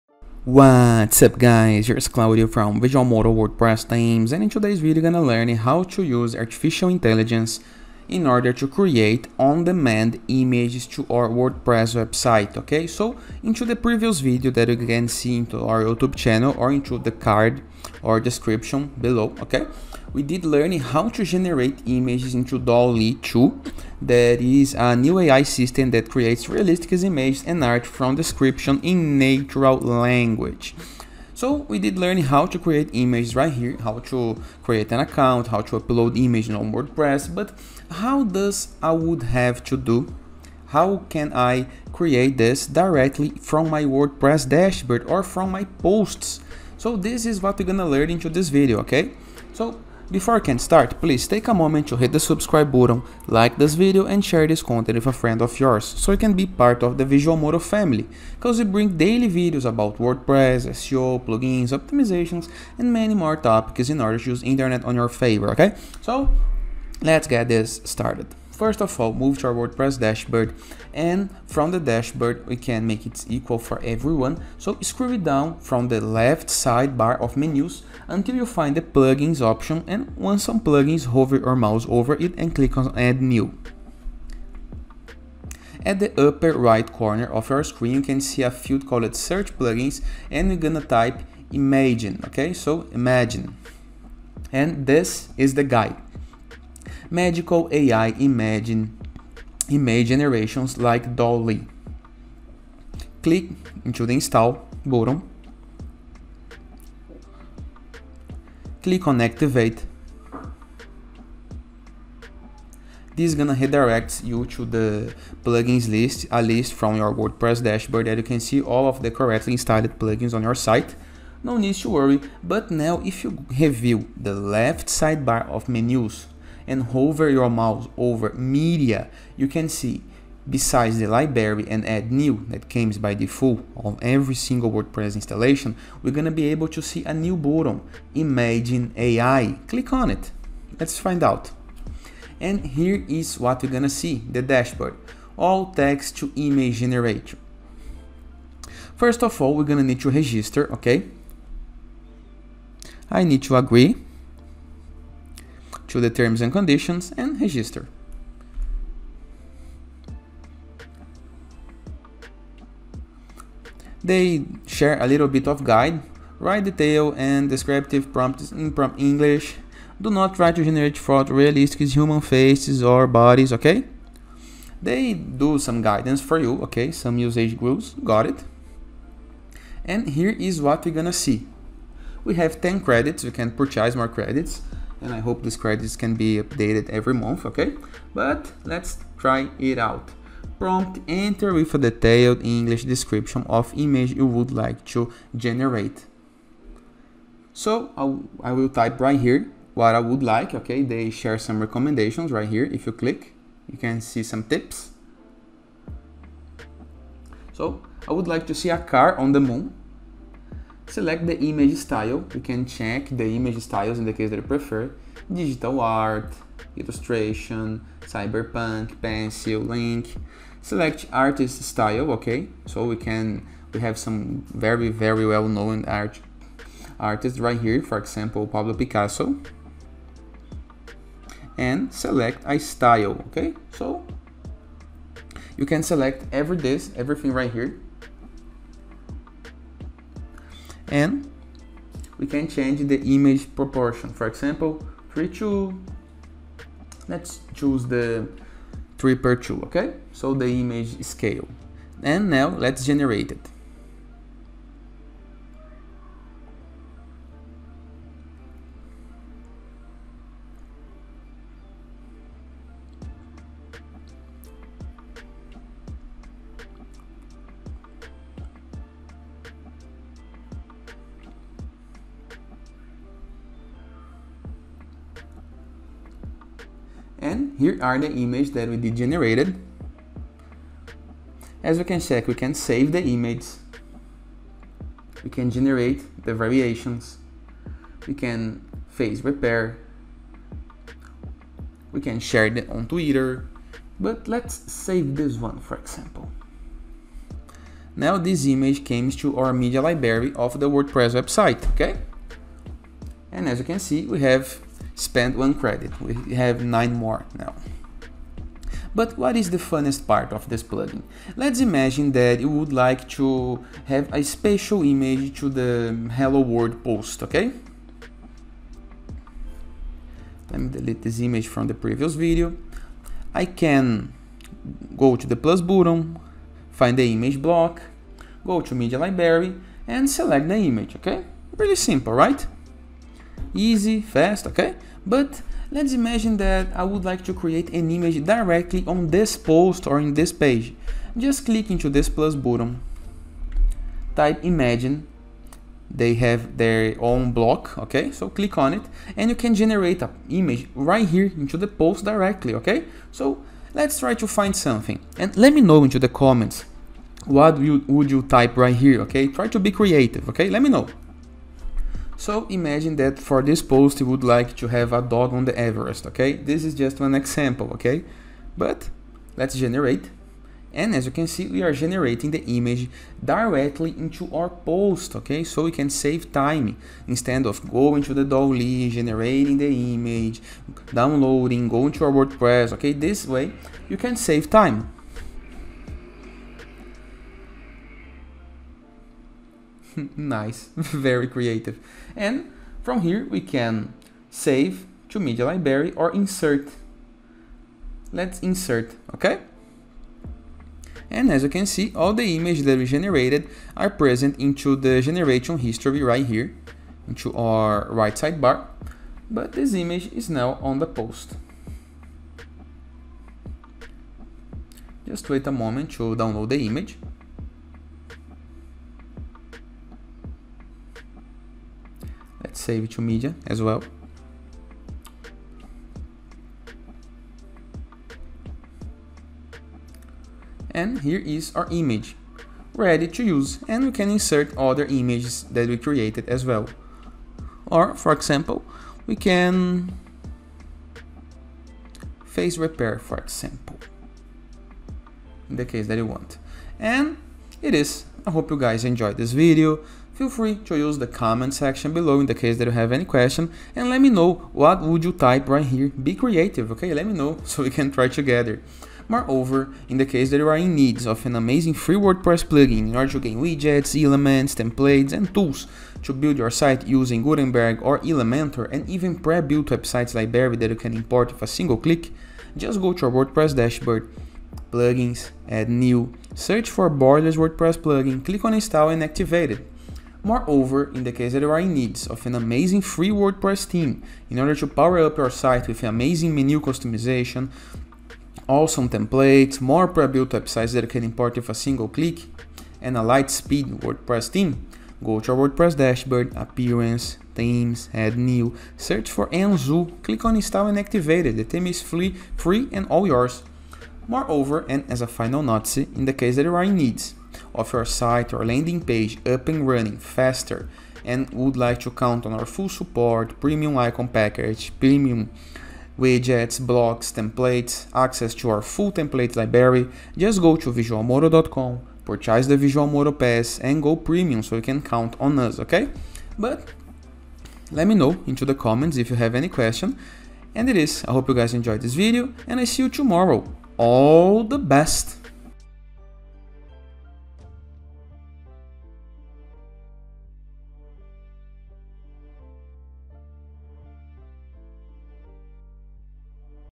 How To Use Imajinn to Generate AI Images in WordPress For Free?
In today's video tutorial we'll learn how to create picture, photos and images on our WordPress site dashboard  using Imajinn to AI for free.

Imajinn is a WordPress plugin that enables the use of AI-generated images on websites. Similar to tools like DALL·E 2, Midjourney, and Stable Diffusion, Imajinn allows you to generate images for any subject with only your imagination. Simply describe what you want to create, and Imajinn will produce a stunning image in a matter of seconds.

Imajinn utilizes cutting-edge artificial intelligence to produce images that are indistinguishable from real photos and art. It's like having a full-time graphic designer who follows your every order. Your only limitation is your imagination.

The base plugin is free and provides 10 credits. After that, you can purchase additional credits from the plugin's website. Upon activation, you must visit the Imajinn AI to configure the plugin settings and generate images. After that, you can begin to generate images using Imajin AI.

Describe the image you want to create. You can also select from a variety of styles, artist styles, style modifier filters, and aspect ratio to further personalize your prompt. Below that, you can choose to click on the Summon button to launch the Prompt Genie tool.

Based on your detailed description, the tool will suggest additional ideas that will produce the greatest possible image. You can utilize the generate button to utilize a suggested prompt, or you can continue writing your own prompt by taking ideas from the suggestions.

Once you're satisfied, click the Generate button to continue. The plugin will now initiate the use of the AI tool on the cloud, this will generate images. After it's complete, it will display four different images for you to choose from.